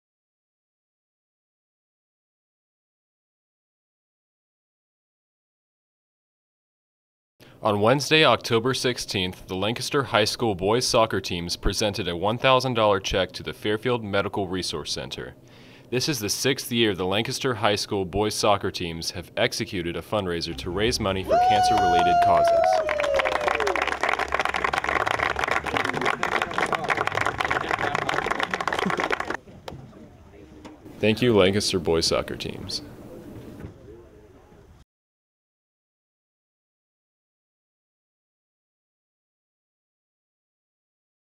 Video of Lancaster High School Soccer Teams Donate $1,000 to Fairfield Medical
This is a short video showing  the LHS Boys Soccer Teams presenting  a $1,000 check to the Fairfield Medical Cancer Resource Center.